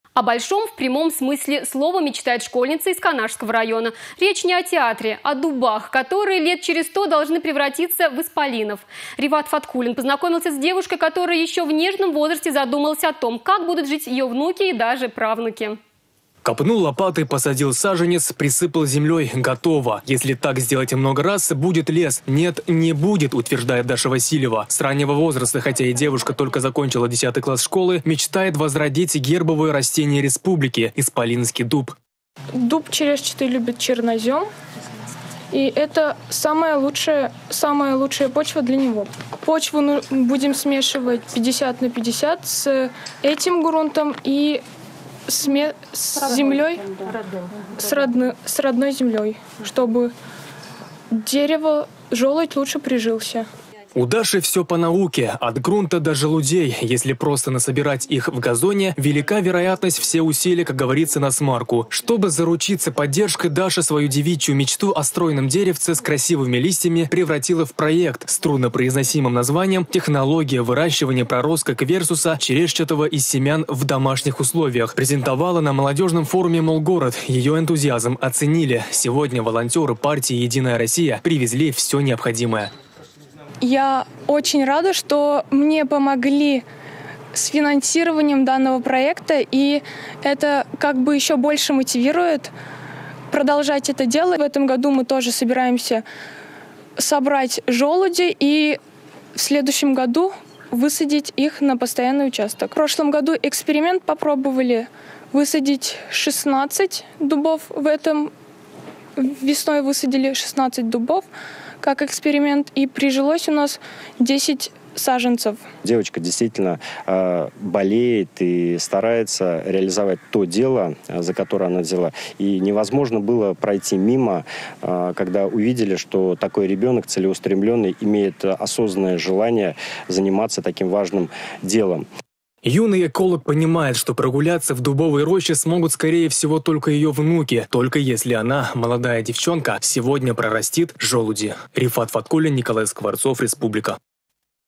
Школьница из Канашского района выращивает дубы
О большом, в прямом смысле слова, мечтает школьница из Канашского района. Речь не о театре, а о дубах, которые лет через сто должны превратиться в исполинов. Рифат Фаткуллин познакомился с девушкой, которая еще в нежном возрасте задумалась о том, как будут жить ее внуки и даже правнуки.